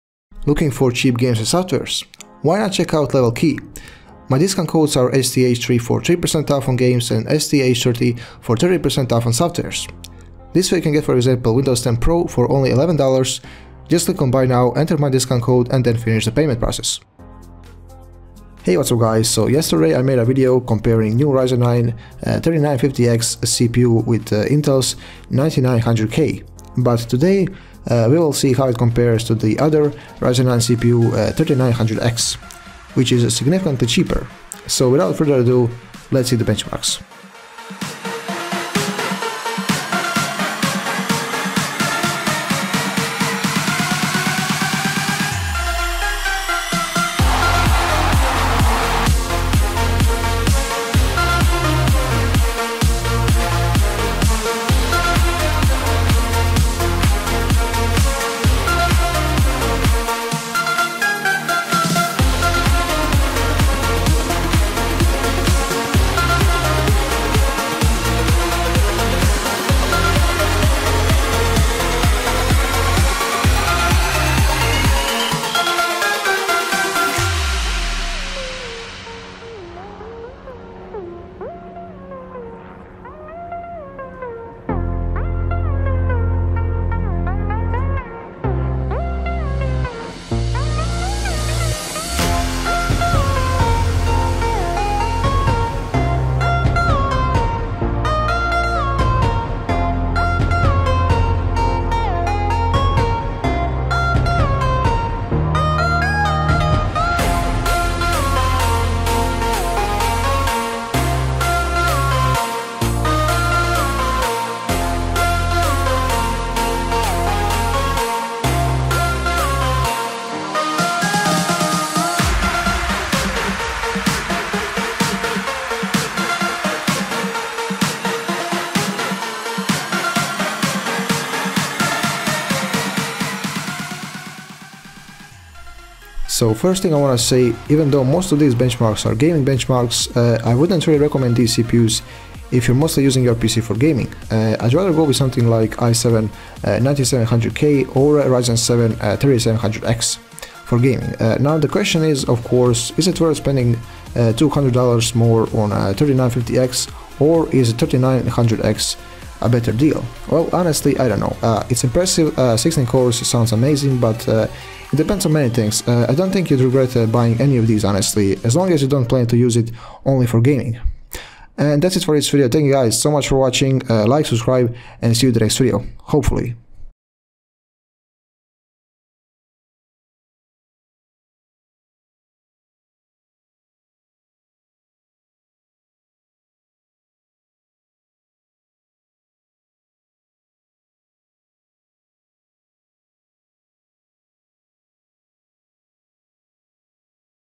Ryzen 9 3950X vs 3900X w/ RTX 2080 (7 Games + Cinebench R20 + Blender)
This video compares Ryzen 9 3950X with 3900X. I really hope you enjoy it. Also, don't hesitate to point out any possible mistakes made in the video on PM or down in the comment section.

TEST SYSTEM:
GPU: NVIDIA GeForce RTX 2080
Motherboard: Asus Crosshair VIII Formula
RAM: 16 GB 3200 MHz DDR4
OS: Windows 10 64-bit